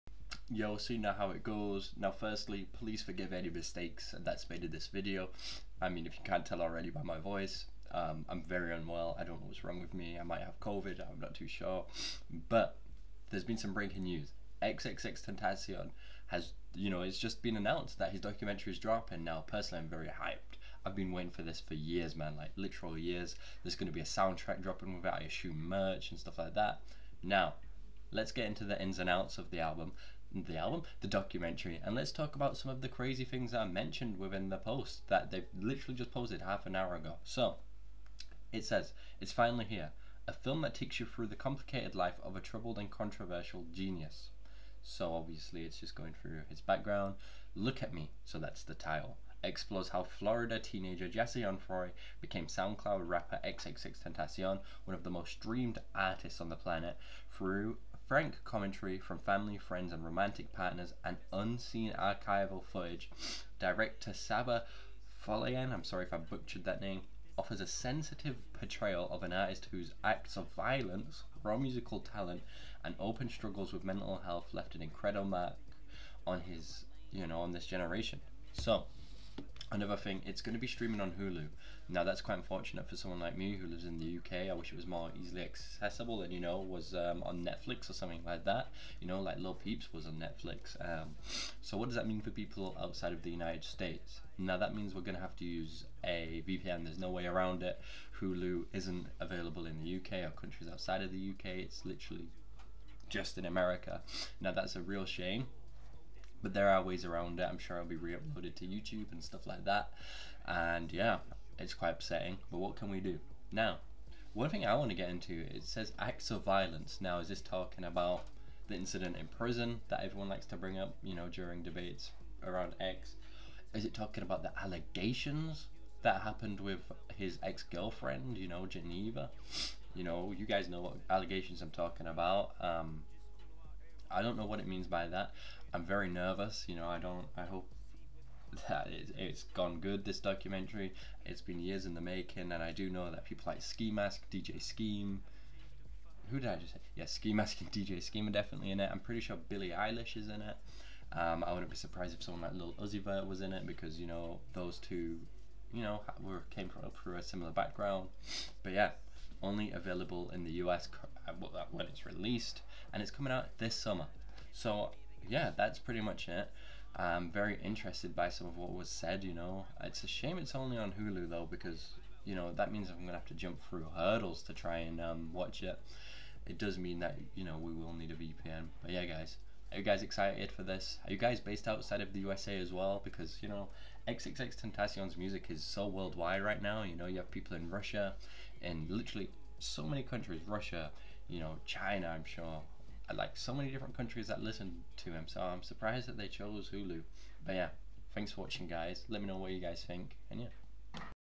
XXXTENTACIONS documentary has been announced
Sorry for any audio issues. I have started using a new setup and video editor.

The longly anticapated XXXTENTACION documentary is finally going to be released this summer on HULU! This is only available in the USA so anyone outside of here needs to use a VPN.

Socials-
Youtube Instagram - @will.couplandyt • Instagram photos and videos